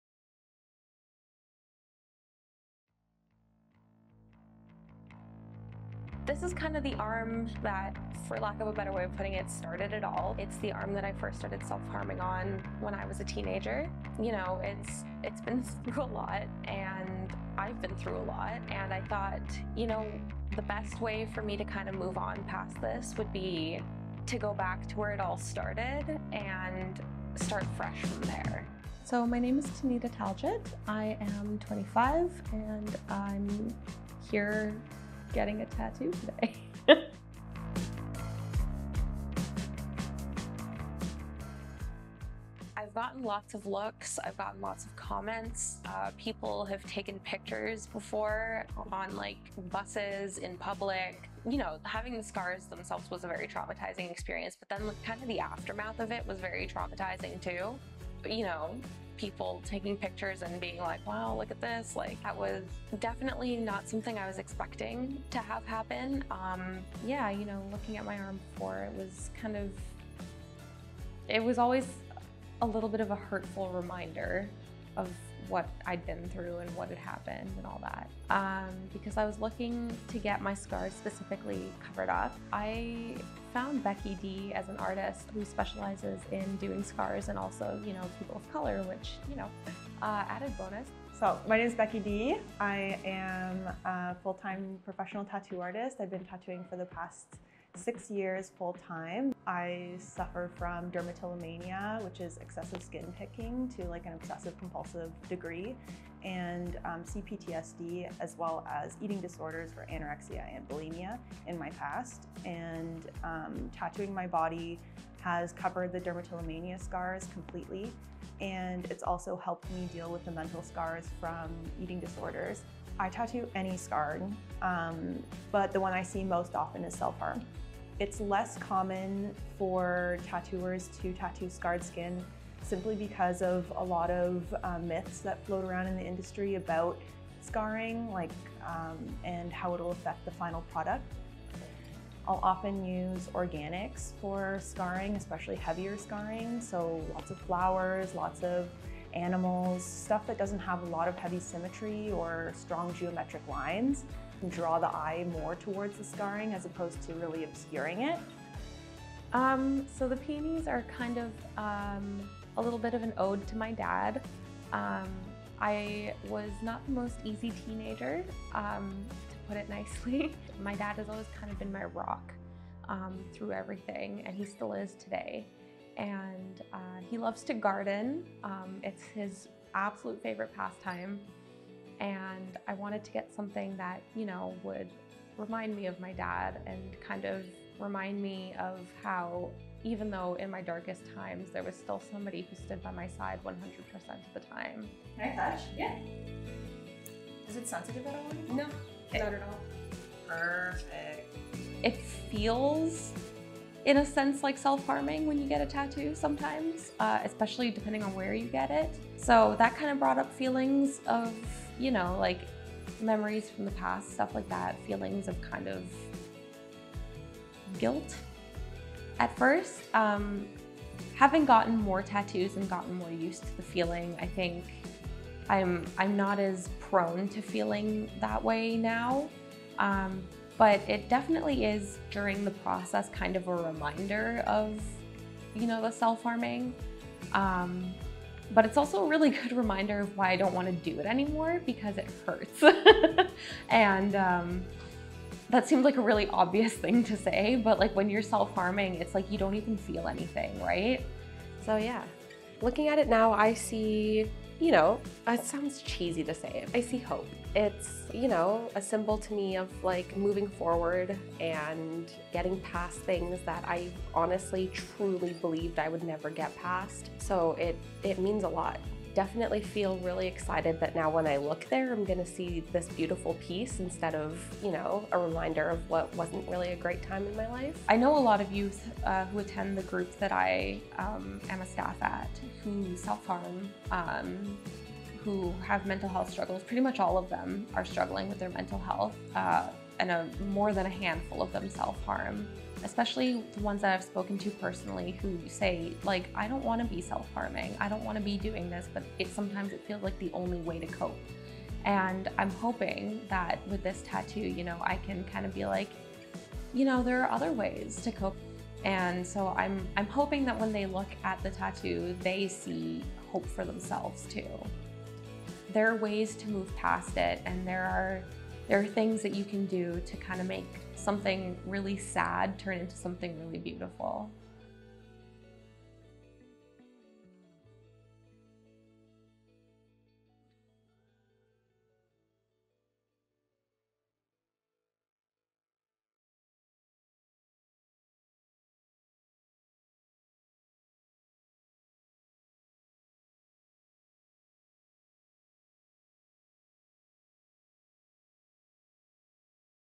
Tattooing over self-harm scars turns pain into art
WARNING: This story contains distressing details.
Taneeta Taljit shares how people reacted to seeing the scars on their arm, and why they sought out Ottawa tattoo artist Becky D to help change something difficult into something beautiful.